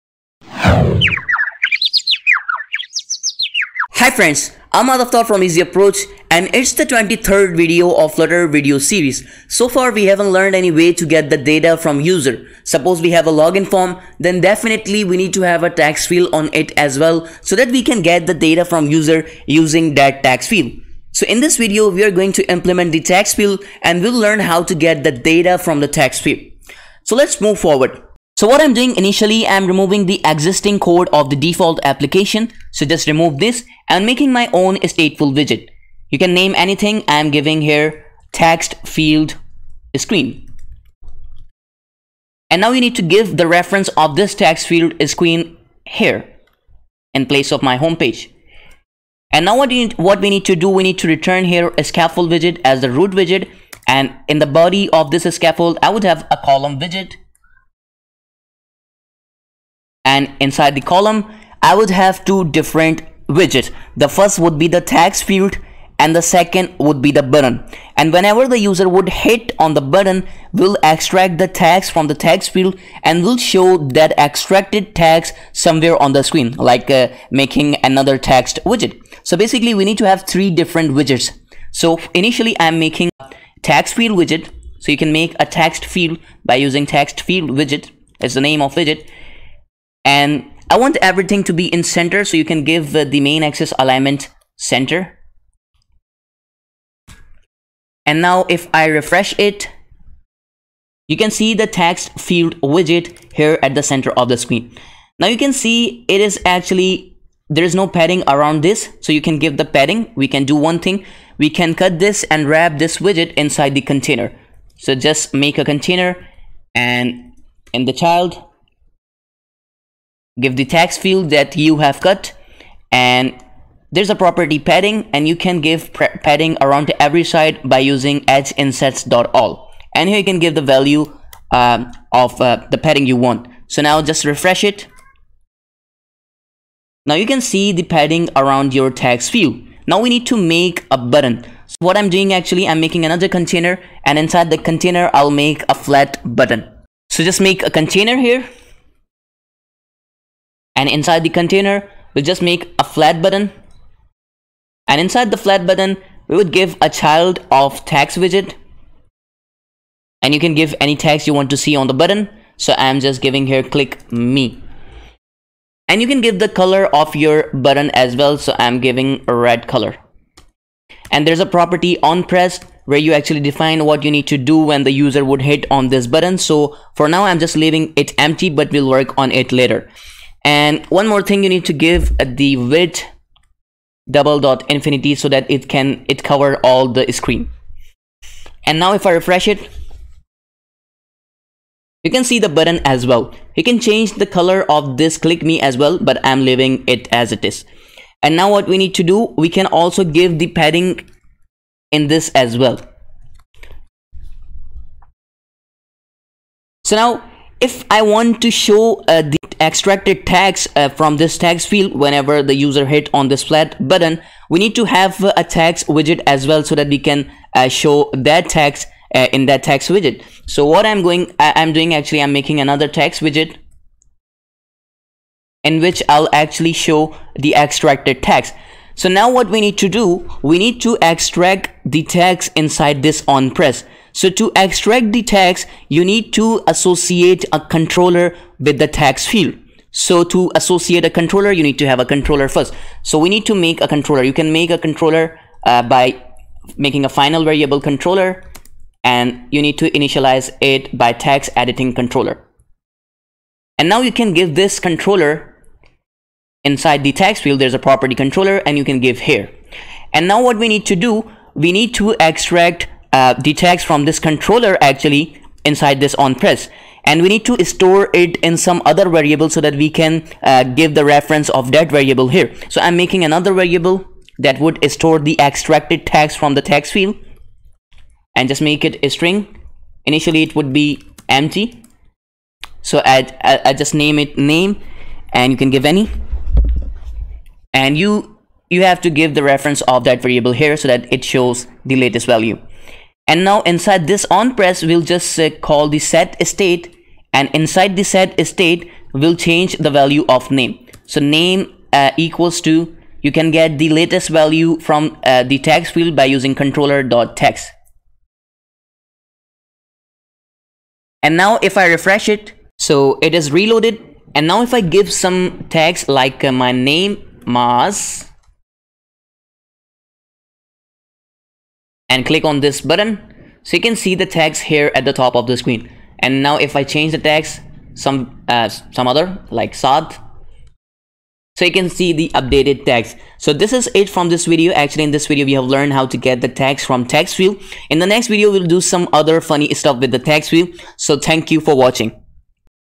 textfield, and text editing controller, flutter video tutorial in English, part 23
In this video, I have discussed about text field, and how to get the text from text field using text editting controller.